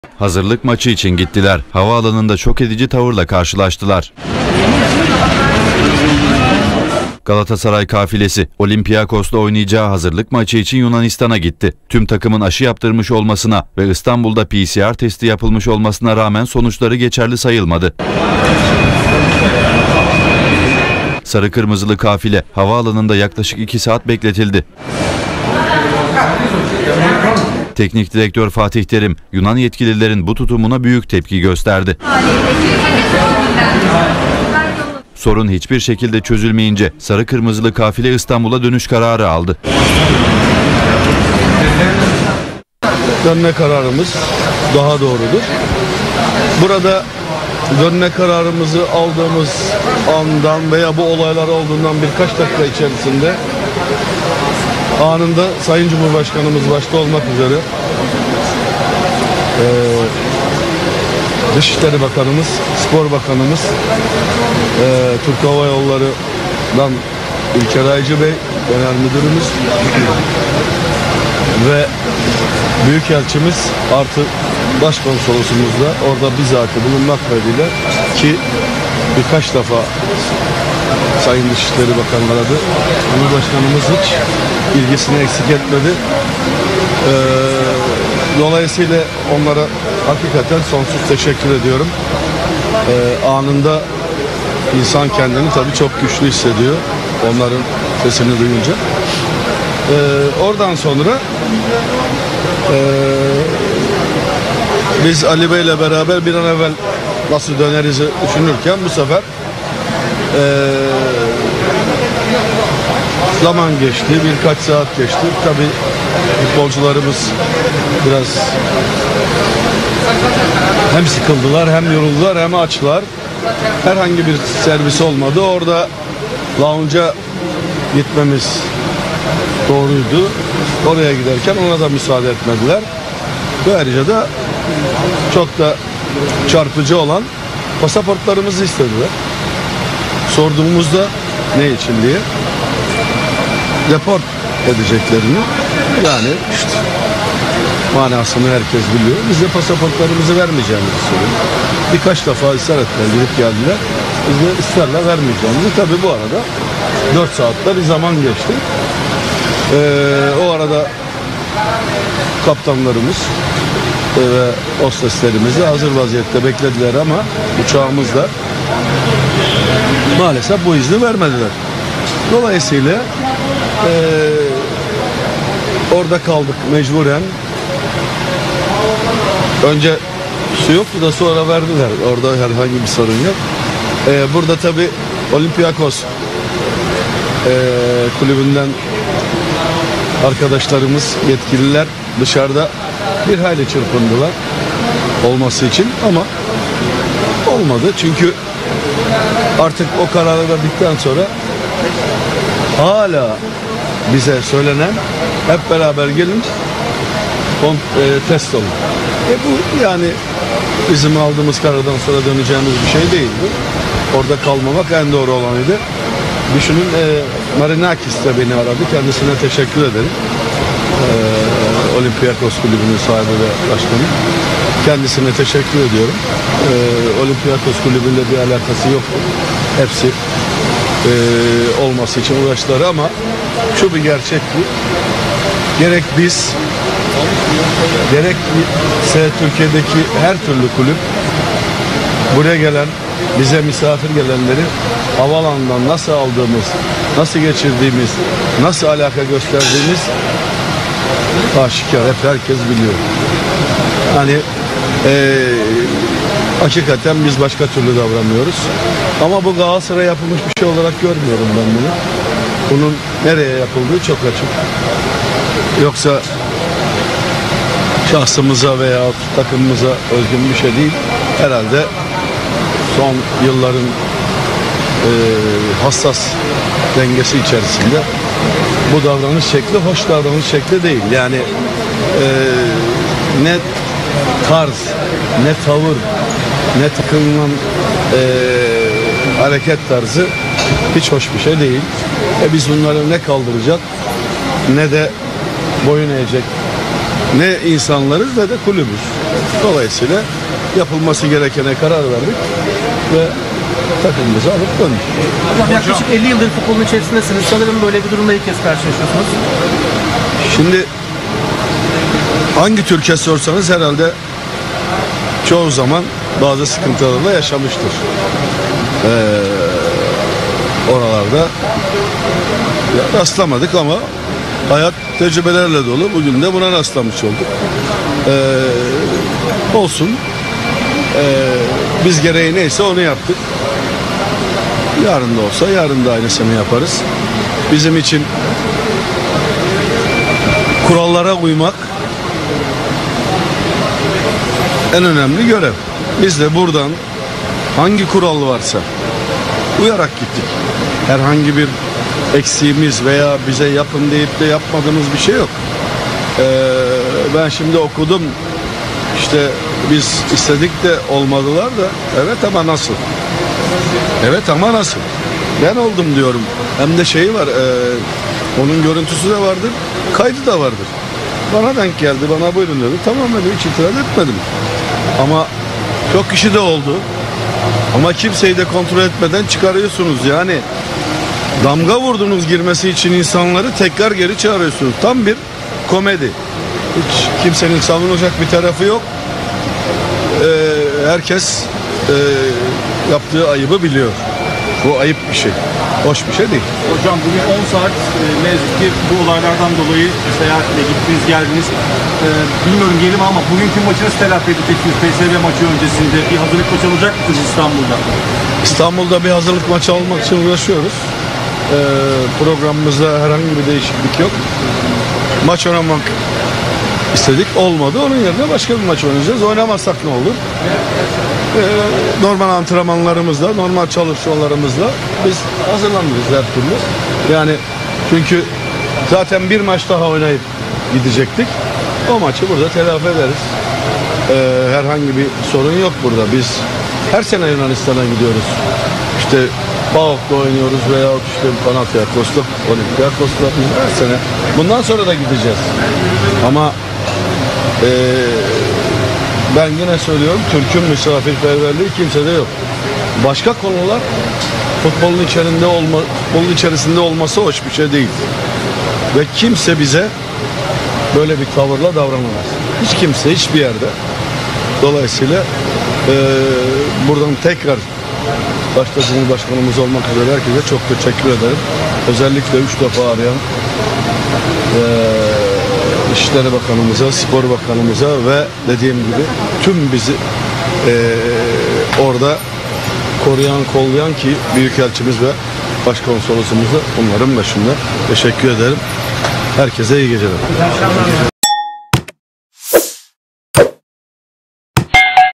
Fatih Terim Yunanistan Skandalını Anlattı! "Dostluk maçları dostlarladır..."
Fatih Terim Yunanistan skandalını bir bir anlattı! "Dostluk maçları dostlarladır..."

Son dakika: Fatih Terim Yunanistan skandalını bir bir anlattı! Dostluk maçları dostlarladır...
Son dakika haberi... Hazırlık maçı için Yunanistan'a giden Galatasaray, Atina Havalimanı'nda Yunan yetkililer tarafından oldukça çirkin bir tavırla karşılaşmıştı. Yaşanan olayların ardından İstanbul'a geri dönmeye karar veren sarı-kırmızılı kafilede Fatih Terim açıklamalarda bulundu. Sert sözlerle yaşanan olayları anlatan Terim, "Dostluk maçları dostlarladır..." diye konuştu.

Olympiakos ile oynanacak hazırlık maçı için Yunanistan'a giden Galatasaray, PCR testlerinin kabul edilmemesi ve Yunan yetkililerin tavırlarından dolayı İstanbul'a dönüş kararı aldı. İstanbul'a dönüşün ardından açıklamalarda bulunan Fatih Terim, Yunan yetkililere çok sert tepki gösterdi.
Yaşanan olayların Olympiakos kulübüyle hiçbir alakasının olmadığını ve onların da mahcup olduğunu belirten Terim, tamamıyla siyasi bir durumun yaşandığını belirtti.

İşte Fatih Terim'in açıklamaları:
Takdir ederseniz ki Galatasaray uluslararası kuralları çok iyi bilen bir kulüptür. Avrupa'da oynanan tüm maçlarda bu tecrübeyi en fazla edinen kulüptür. 24 saat önce hepimiz test olduk. Testlerin belgeleriyle beraber oraya gittik.

"DOSTLUK MAÇI DOSTLARLADIR"

İndiğimizde hızlı bir testin yapılmasını istediler. Son iki gündür bunu uygulamaya başladıklarını, herkes için değil rastgele olduğunu söylediler. Ben de test oldum. Elimizdeki PCR testlerinin de fotoğrafını çekiyorlar. Bir kısmına ihtiyaç yok dediler, onlar dışarı çıktılar.
Dostluk maçları dostlarladır. Yapılan davranış, dostça bir davranış değil. Dönme kararımızın daha doğru olduğunu düşündük. Cumhurbaşkanımız ilgisini hiç eksik etmedi. Onlara sonsuz teşekkür ediyorum. Onların sesini duyunca insan kendisini çok güçlü hissediyor.
Nasıl döneriz diye düşünürken, futbolcularımız biraz sıkıldılar, yoruldular, hem de açlar. Herhangi bir servis olmadı. Pasaportlarımızı istediler. Sorduğumuzda, deport edeceklerini söylediler. Biz de pasaportlarımızı vermeyeceğimizi söyledik.

"BU YAPILANLAR GALATASARAY'A KARŞI DEĞİL"
Olympiakos kulübünden yetkililer bir hayli çırpındılar olması için. Ama olmadı. Bize hala söylenen "Hep beraber gelin, test olun." Bu karardan sonra orada kalmamız doğru olmazdı.
Bu yapılanları Galatasaray'a karşı yapılmış olarak görmüyorum. Bunun nereye yapıldığı açık. Hiç kimse bize böyle bir tavırla davranamaz.

İstanbul'da bir hazırlık maçı oynamak için uğraşıyoruz. Programımızda bir değişiklik olmayacak. Bir maç oynayıp gidecektik, o maçı burada telafi edeceğiz.